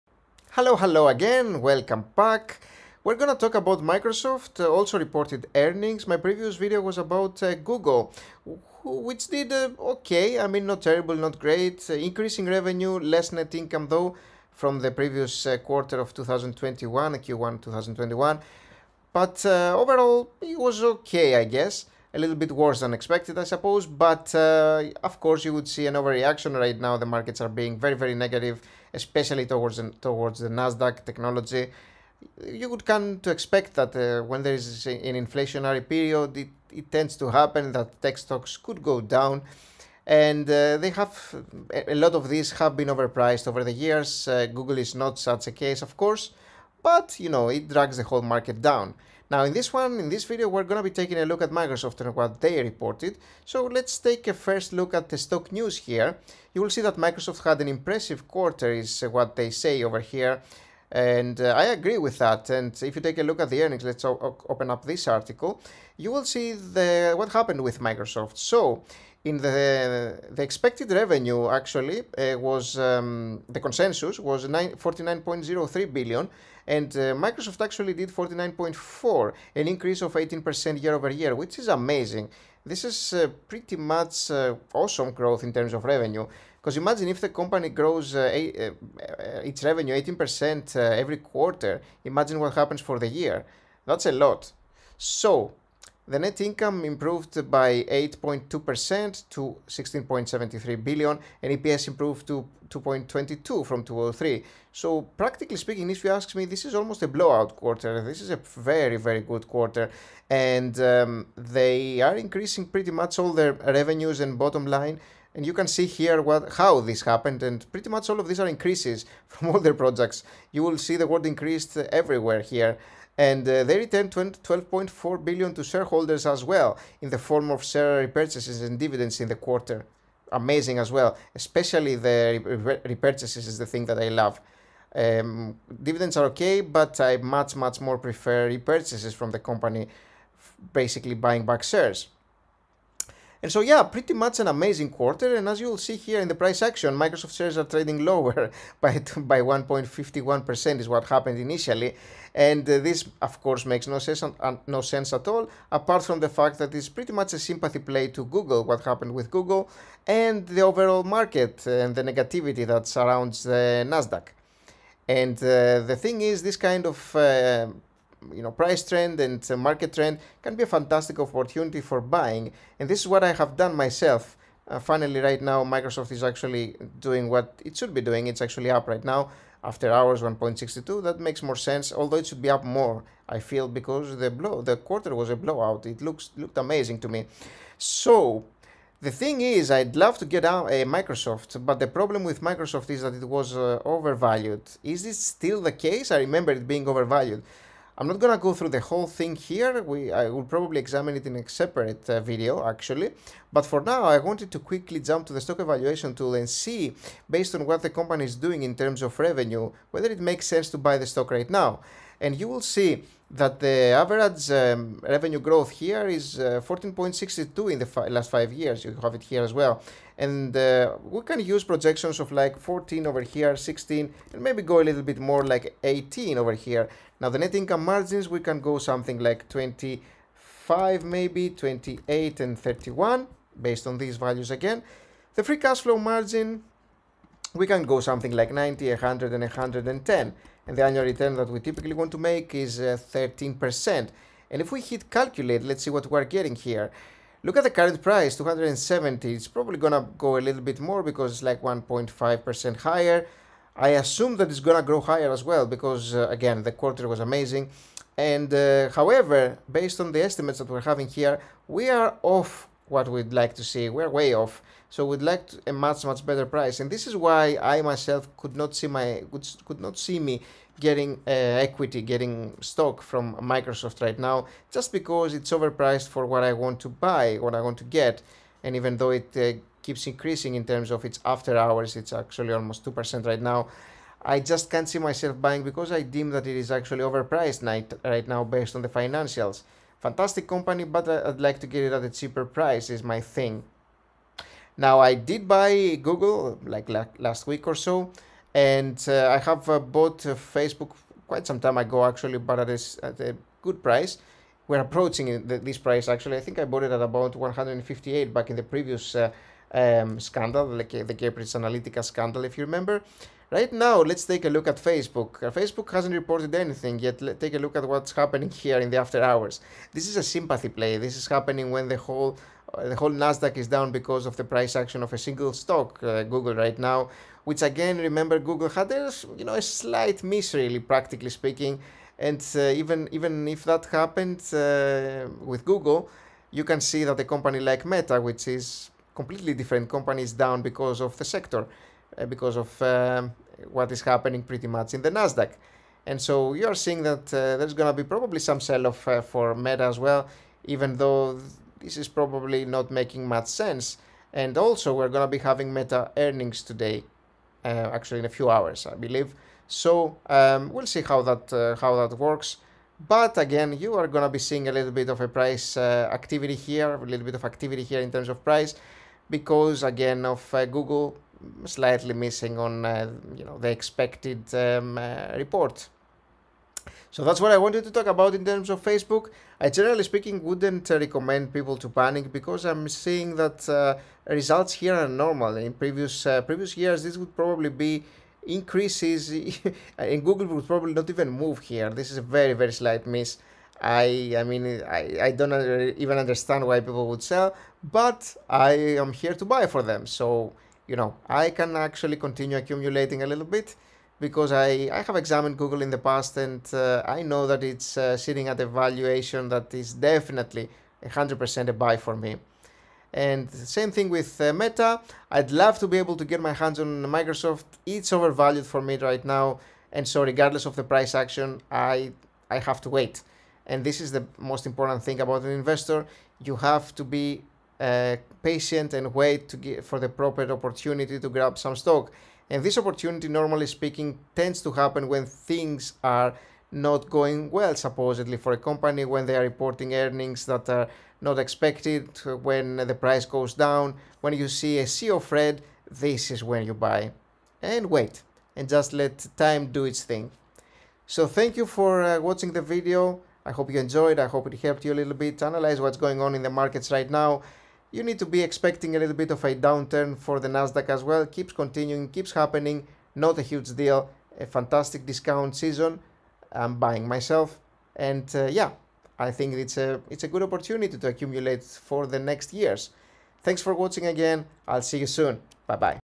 Microsoft Stock Great Earnings Report ! Is it a BUY ? | MSFT Stock
Microsoft had a pretty good earnings report just now. I am worried that the stock is still a bit overvalued though. Is this the case? Let's take a look.